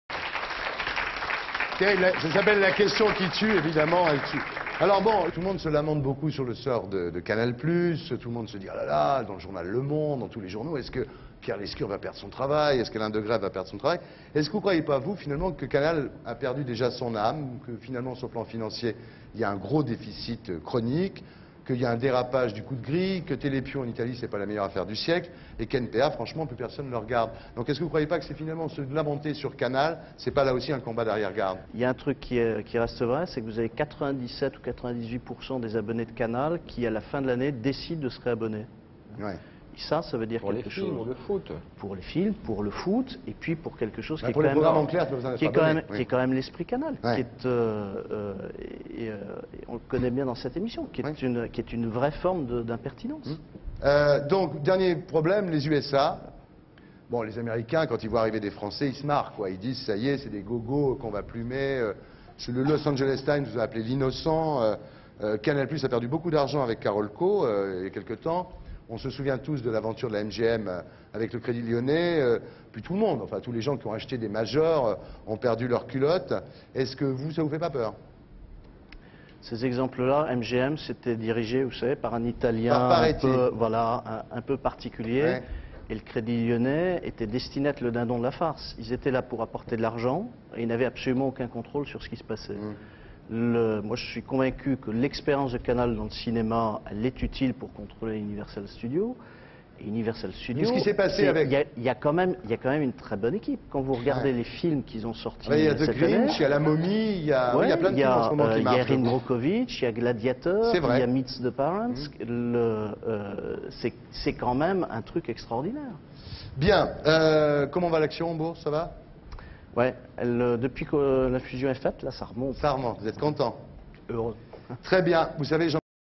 Jean-Marie Messier "L'esprit Canal +" - Archive INA
16 décembre 2000
à propos de l'esprit "canal plus", une vraie forme d'impertinence.Les déboires industriels et commerciaux de Vivendi aux Usa.L'action en bourse remonte, JMM est "heu-reux" Images d'archive INA